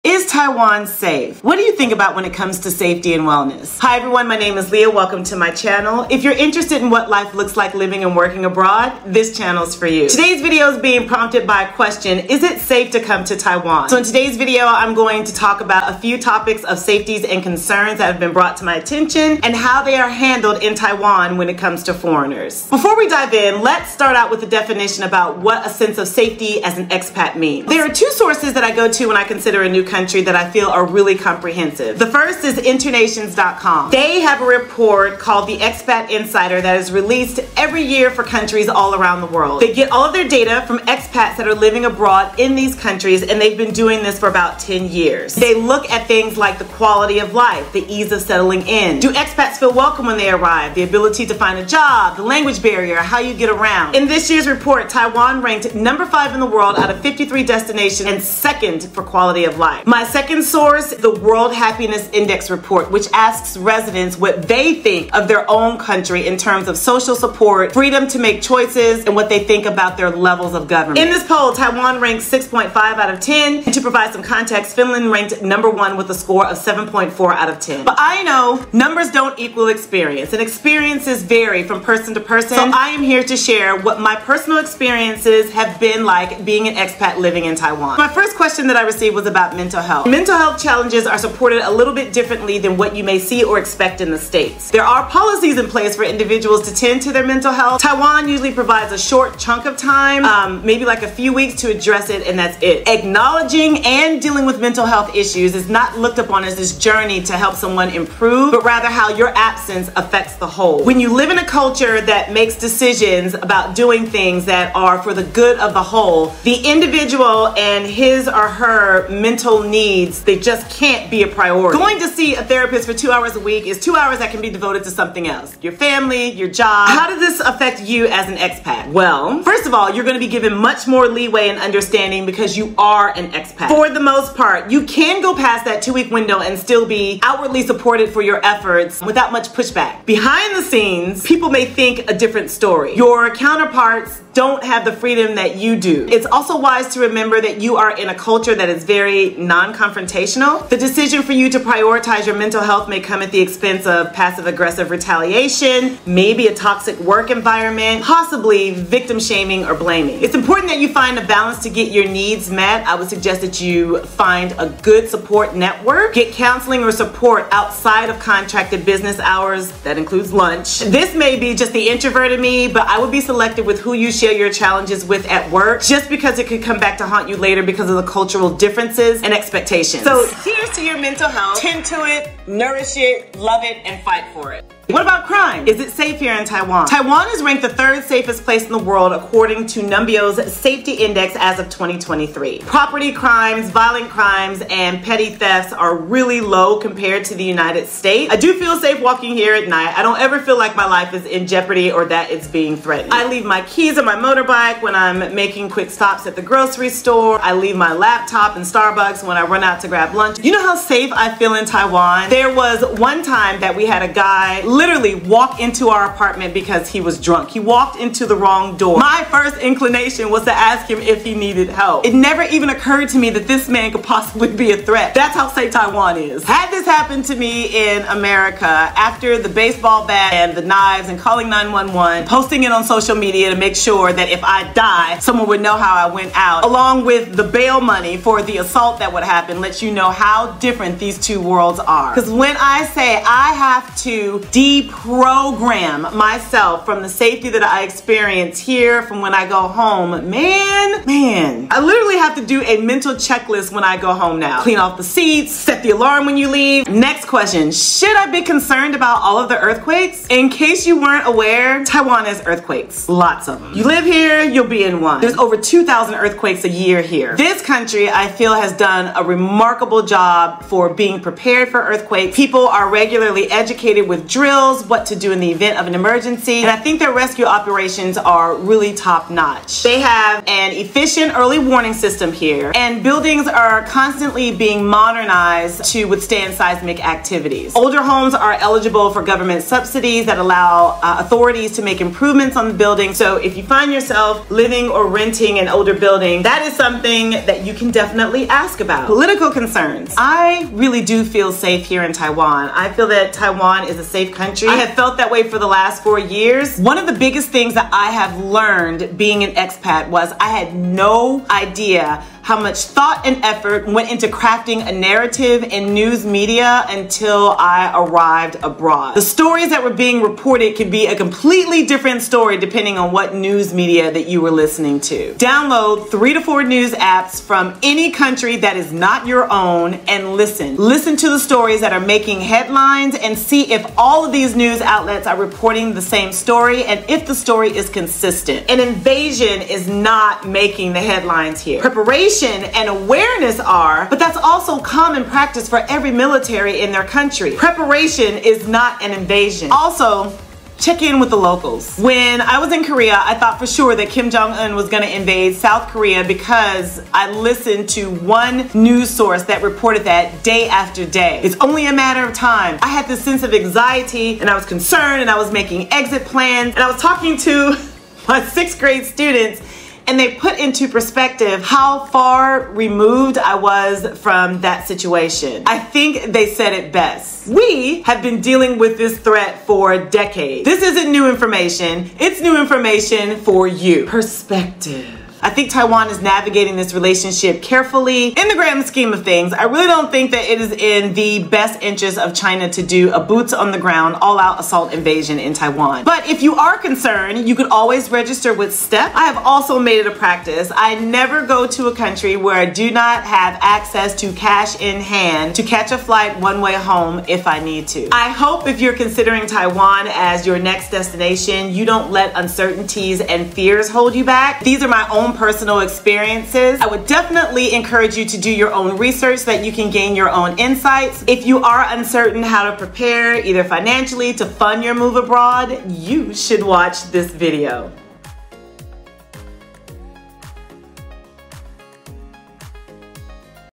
Is It Safe For Expats To Come To Taiwan?
In today's video, we're discussing topics that expats want to know about relocating to Taiwan  including safety, culture, and more. Tune in to get the info you need.

TIMESTAMPS:
00:00 - 00:23 - Intro
00:24 - 01:27 - DATA For You
01:28 - 03:03 - MENTAL HEALTH Health For Expats
03:04 - 04:17 - What's CRIME Like In Taiwan
04:14 - 04:59 - Should You Be Concerned About EARTHQUAKES
05:00 - 07:13 - ATTACK on Taiwan